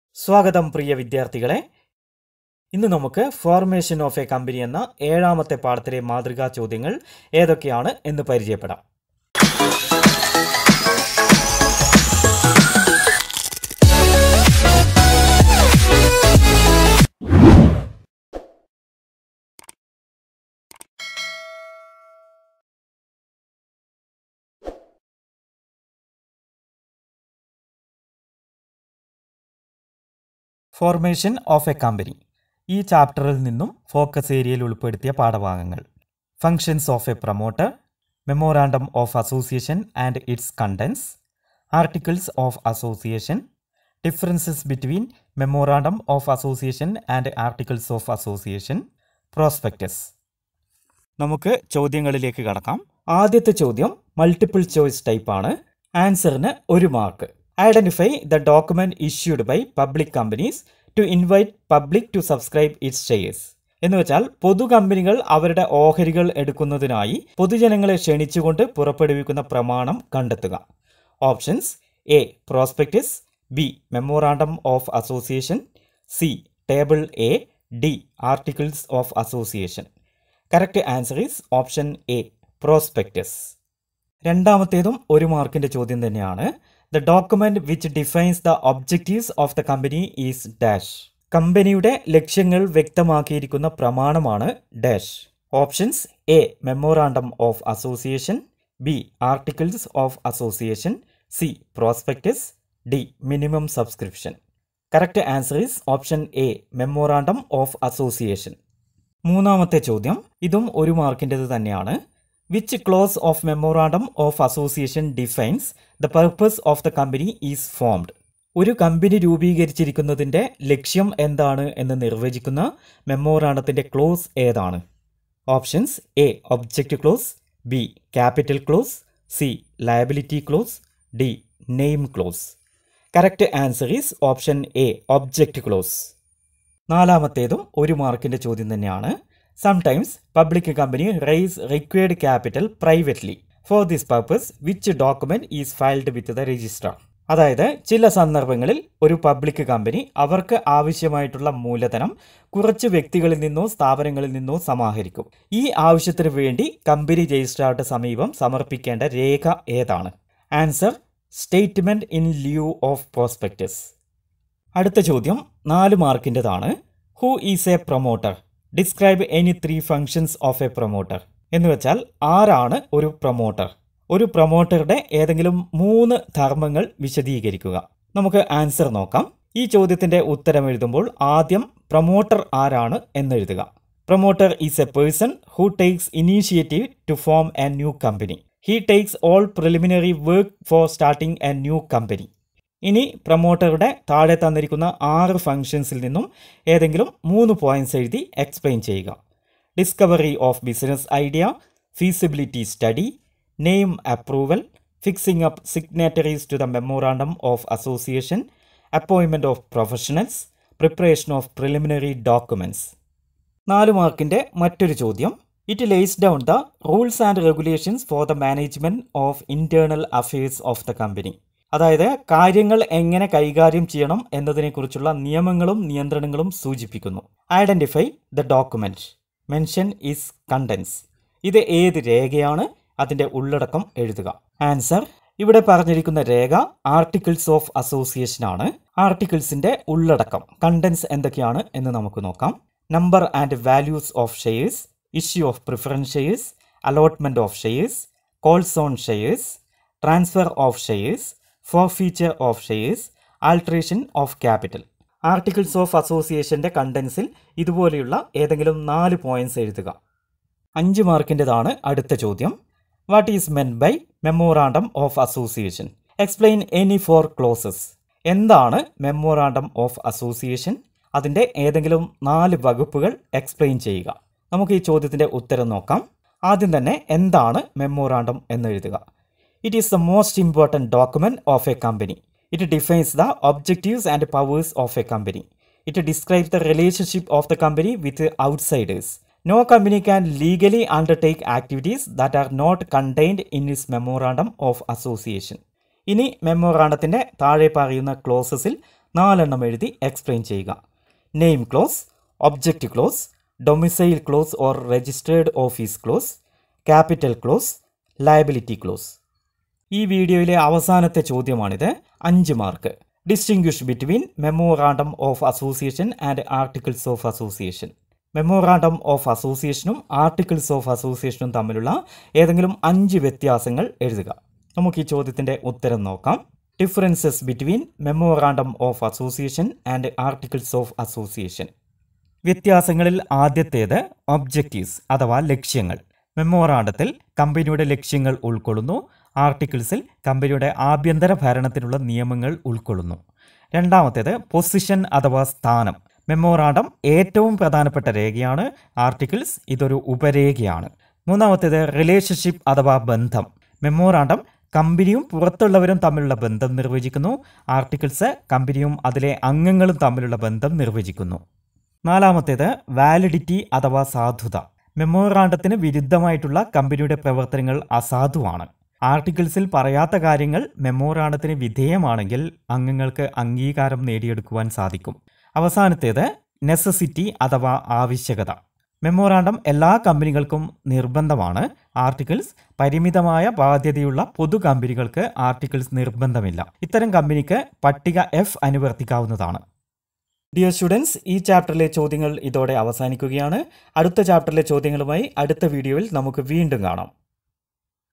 XI Business Studies |Focus Area Questions and Answers | Chapter 7 | Formation of a Company
Sample Questions from Focus Area
Chapter 7 Formation of a Company

Malayalam and English Notes:
മലയാളത്തിലും ഇംഗ്ലീഷിലുമുള്ള നോട്ടുകൾക്ക് സന്ദർശിക്കുക: